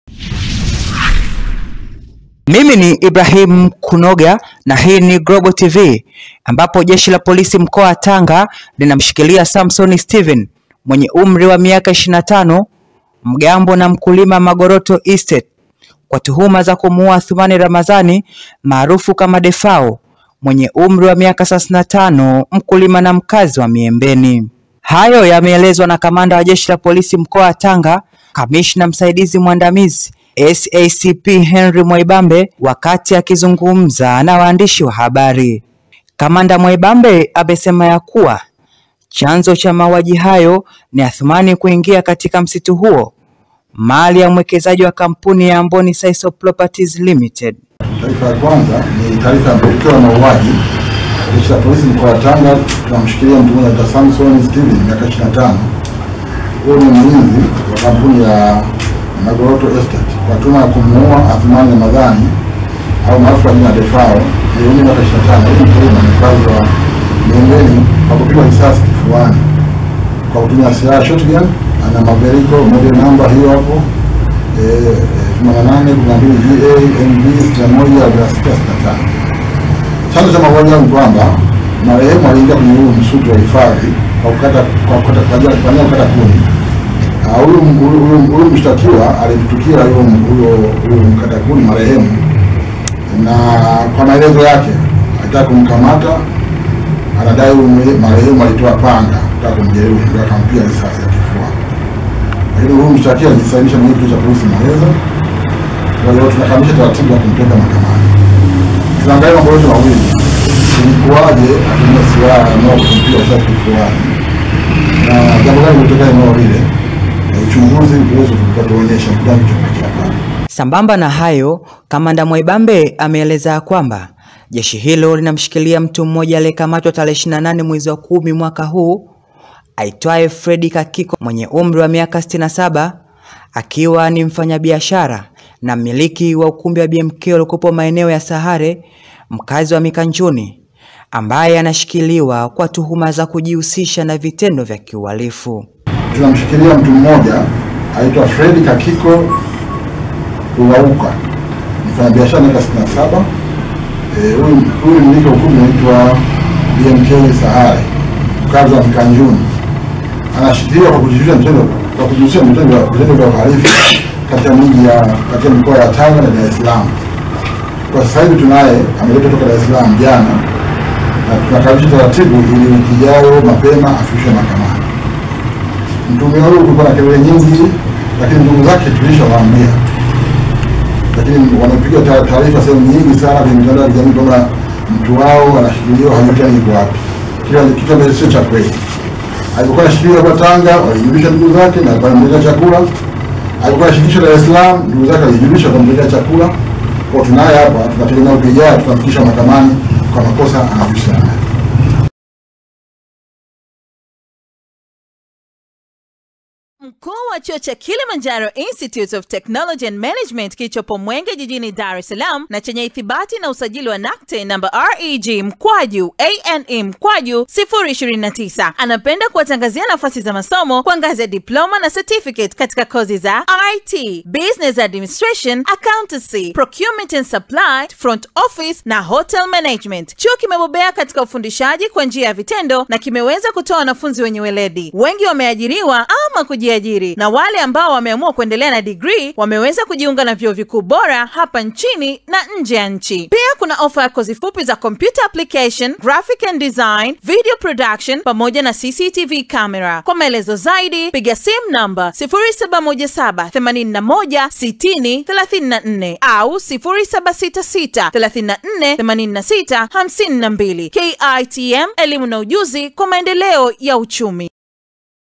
DEFAO WA TANGA AUAWA KWA KUPIGWA RISASI YA KIFUANI NA MGAMBO, KAMANDA AELEZA - "ALIINGIA MSITUNI"
Jeshi la Pilisi mkoani tanga linamshikilia mtu mmoja Samson Steven (25) kwa tuhuma za kumuua kwa kumpiga risasi Athumani Ramdhani maarufu kama Defao.